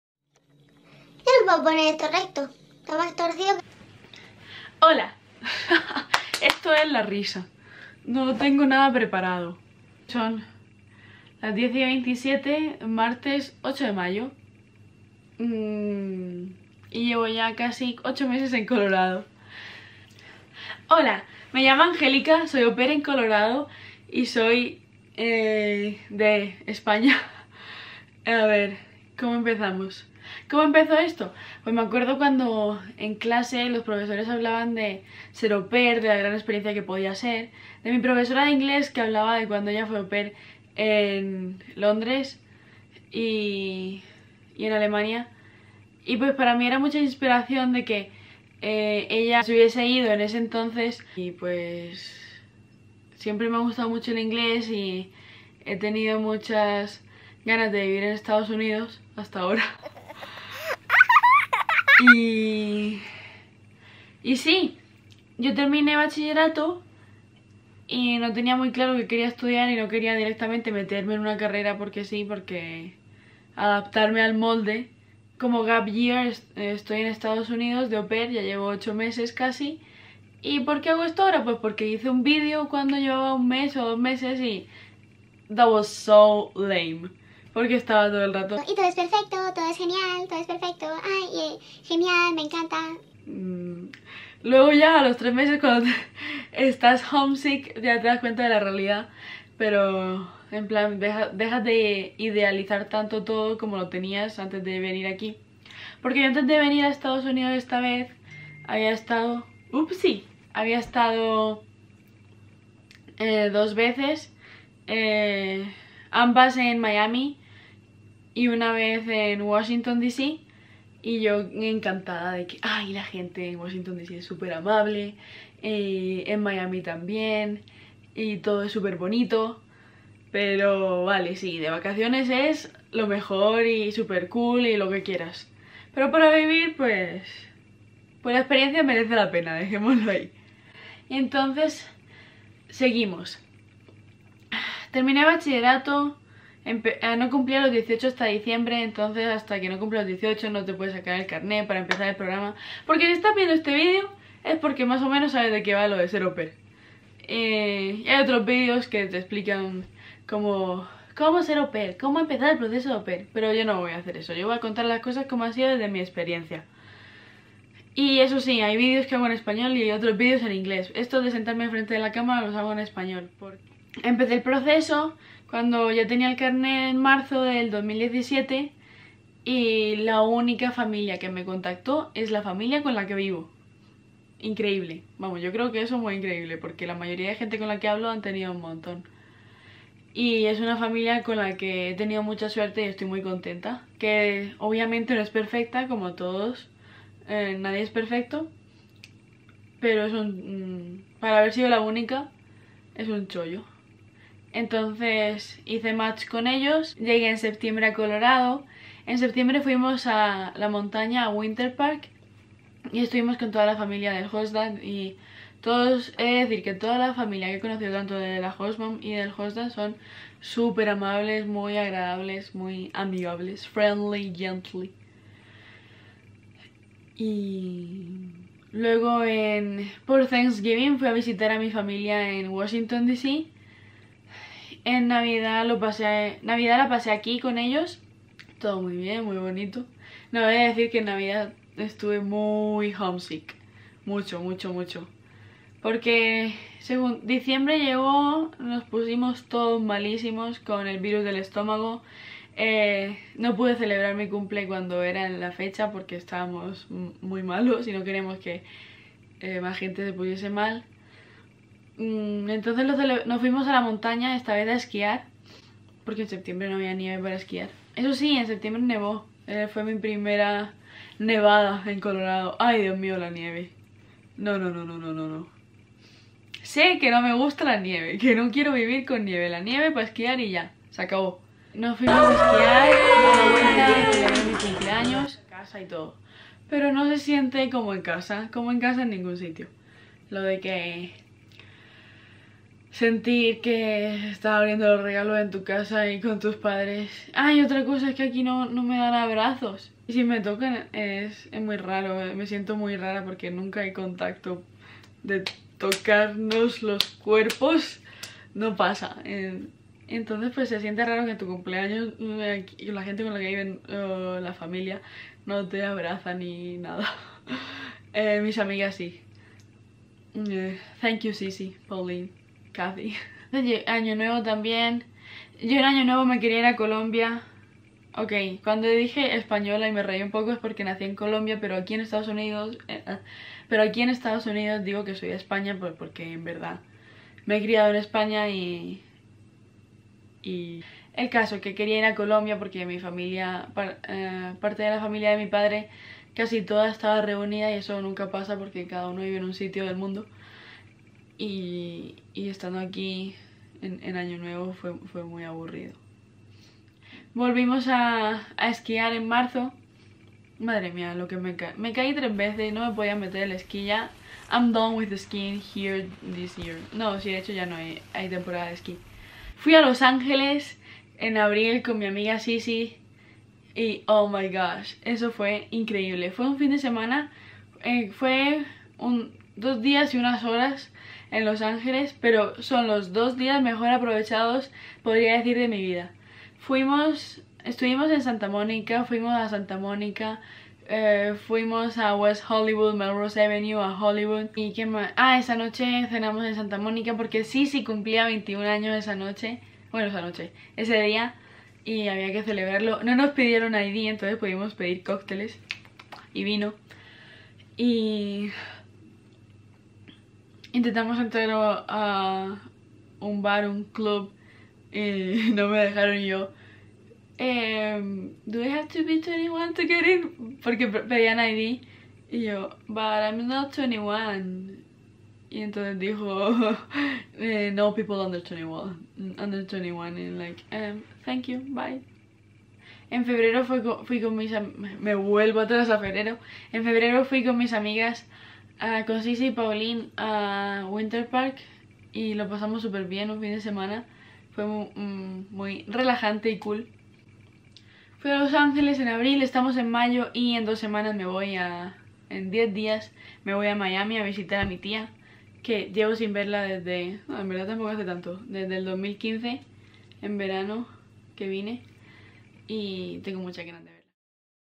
8 meses en USA| presentación. ¿Qué he hecho en 8 meses y como surgió la idea?
Hola!! Aquí os cuento como surgió la idea de ser au pair y algunas de las cosas que he hecho en estos 8 meses.
Atentos que hay una segunda parte!